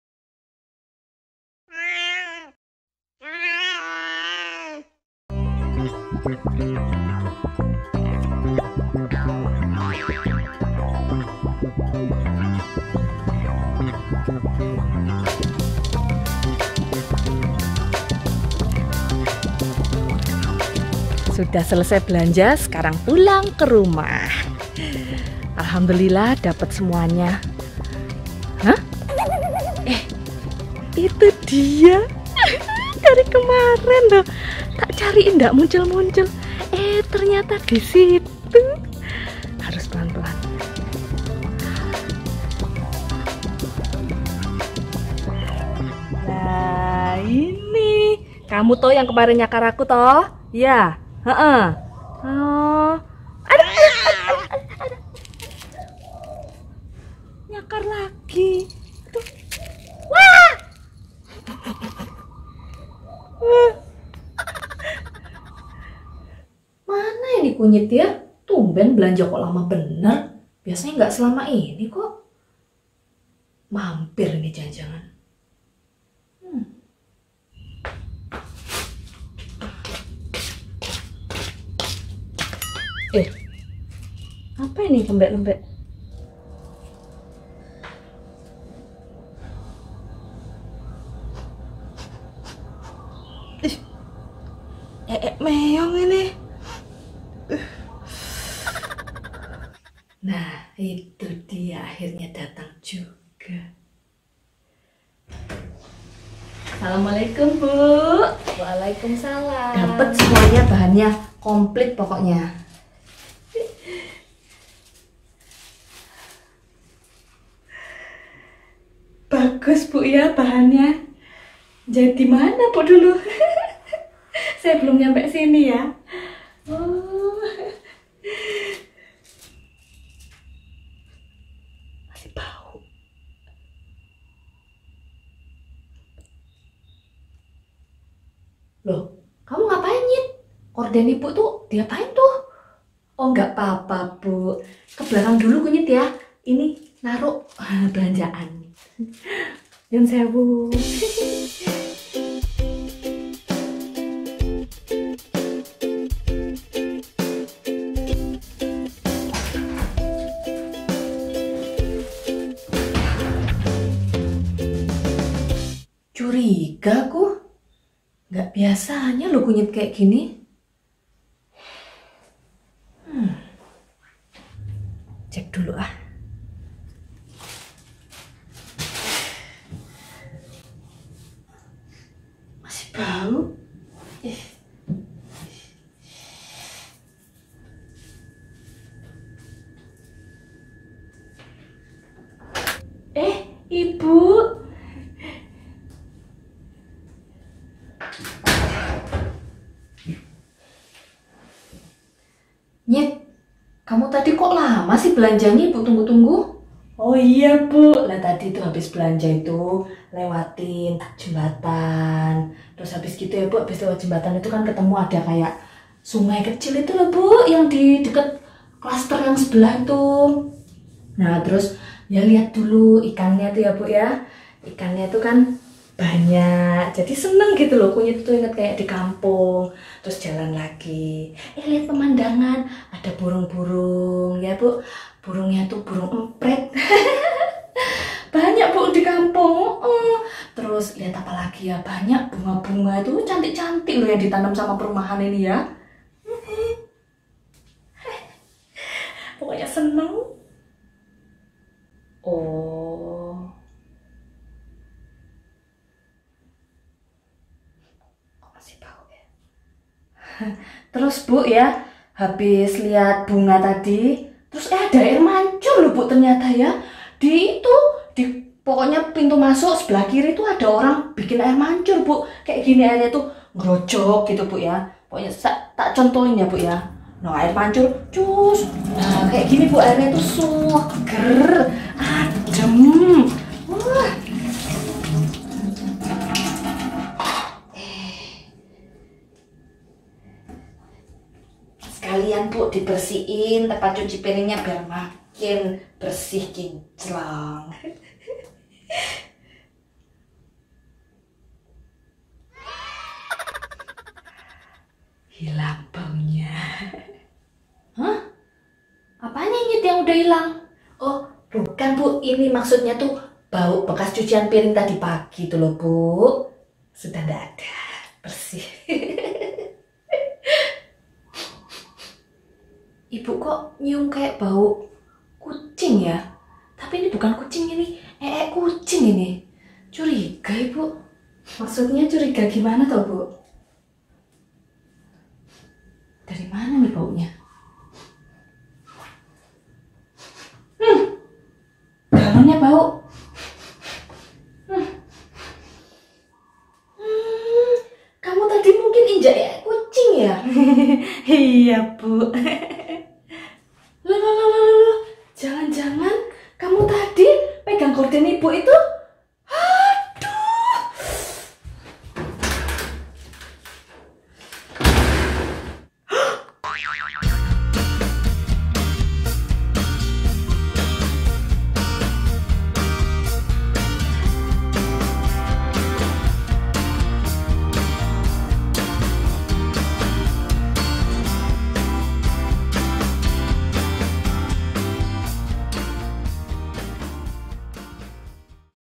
Kunyit Dapat Hadiah Spesial Dari Si Kucing Meong 😄 Asti Kunyit - Bunda Aqilla Eps 49
Sepulang Kunyit dari berbelanja, ia bertemu dengan makhluk berbulu yang pernah mencakarnya beberapa hari lalu. Siapa lagi kalau bukan si Meong kucing.

Dengan rasa gemas ia mencoba menangkap, namun sekali lagi si kucing berontak dan mencakar tangan Kunyit. Setelah berhasil kabur, ternyata Kunyit malah mendapat hadiah spesial dari Si Meong. Apakah itu..??

Saksikan video ini sampai selesai ya, lucu dan menghibur lho...